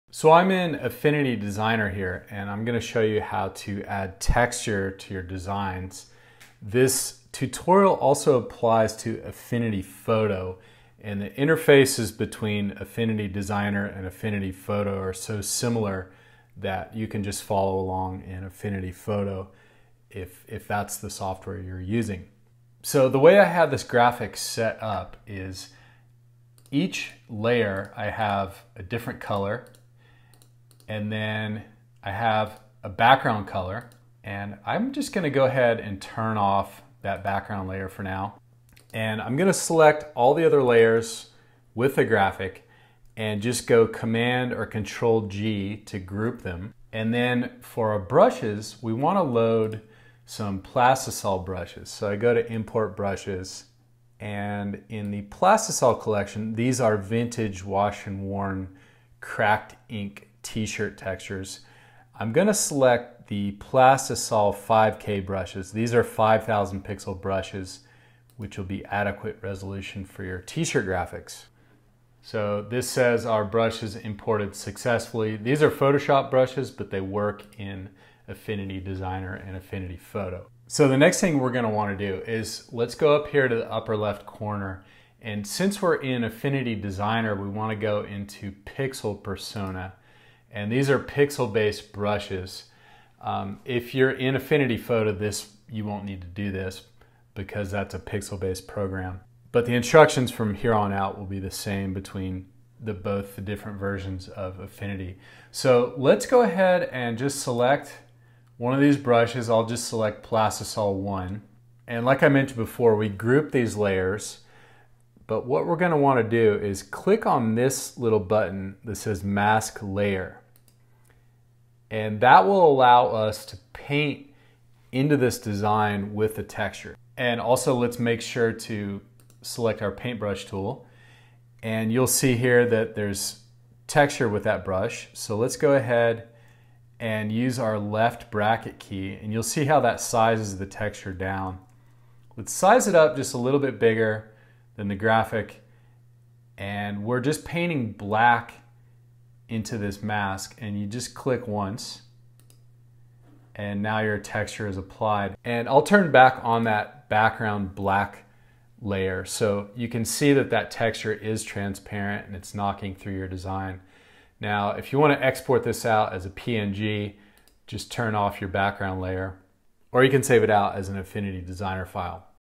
How to Apply Texture in Affinity Designer and Affinity Photo
Apply transparent t-shirt texture using Photoshop Brushes in Affinity. The textures shown are from the Plastisol Collection